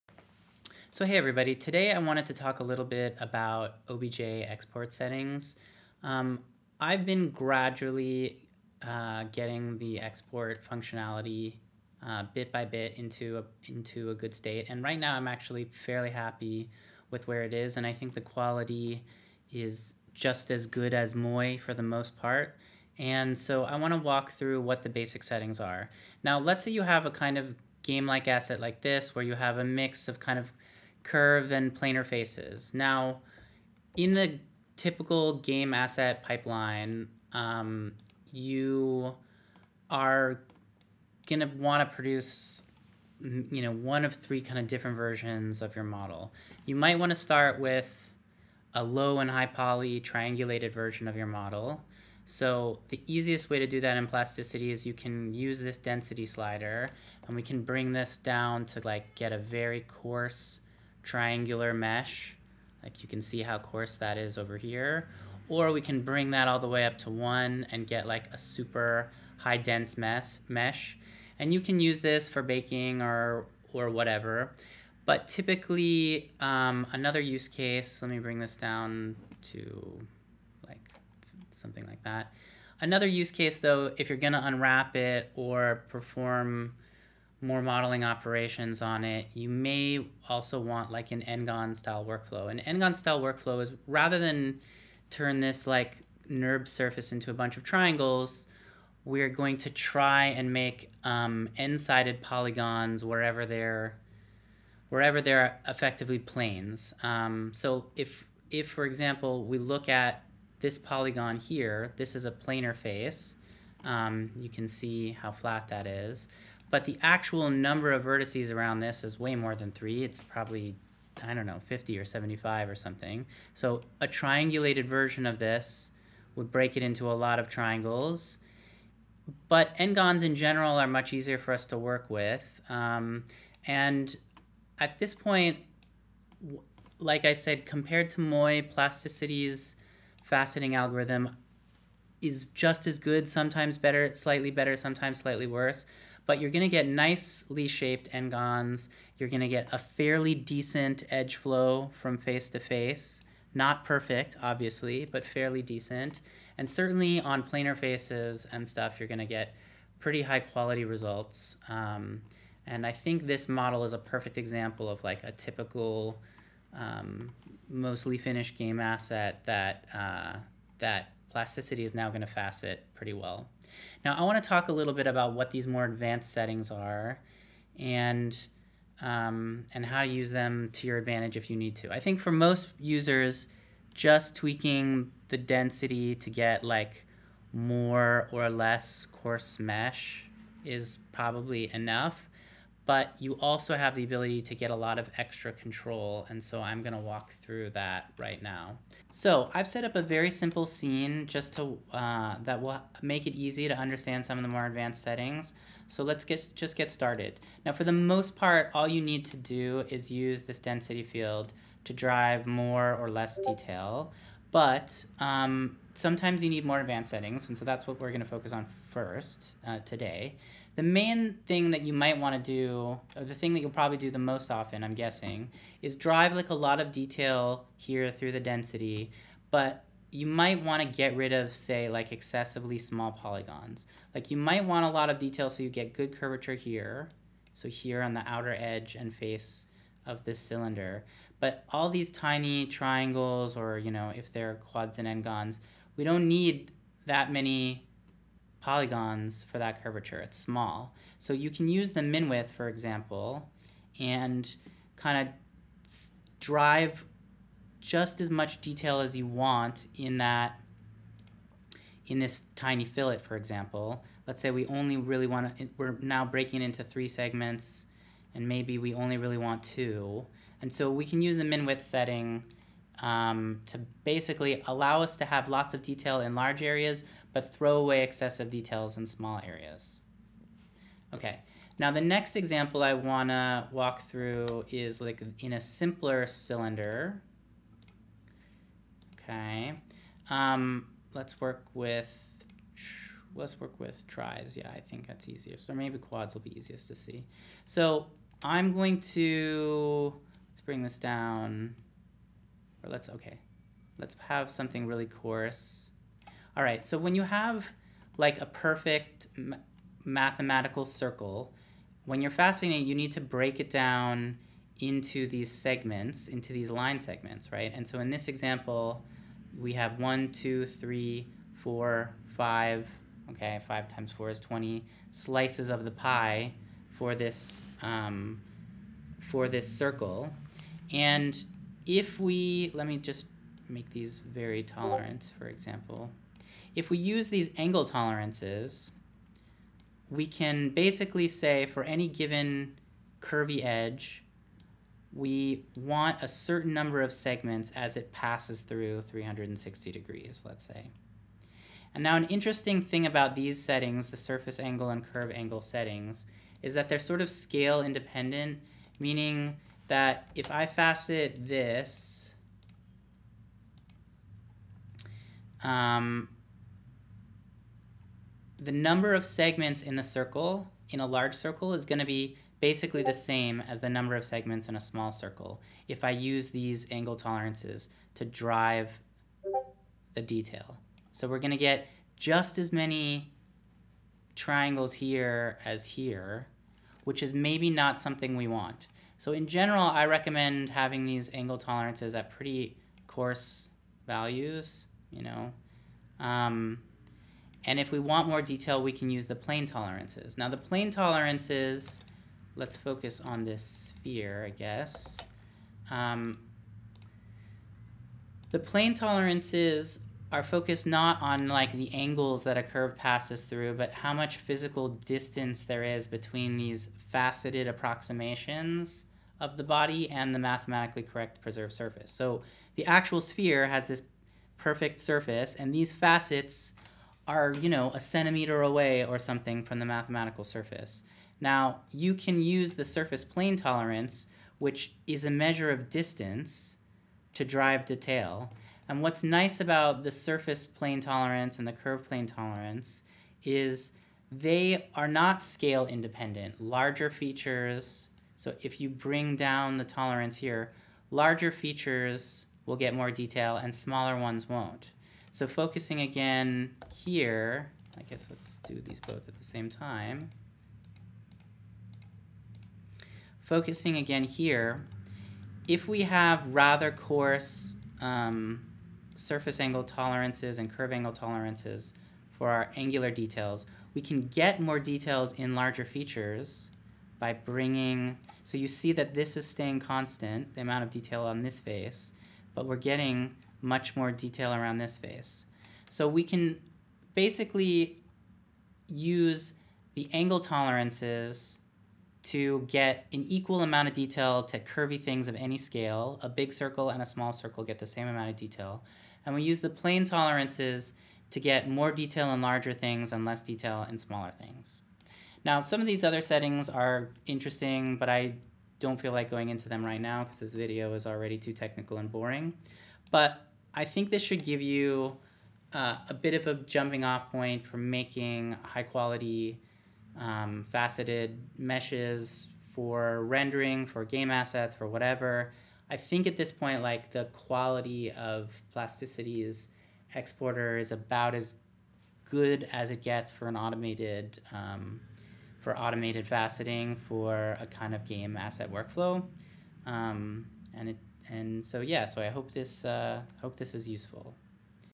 Exporting meshes (triangles, ngons, etc.) from plasticity
Plasticity’s OBJ exporter has gotten better and better. Now you can export very high quality trangular/ngon meshes. The purpose of this video is to show off the kind of quality you can expect and how to use some of the more advanced settings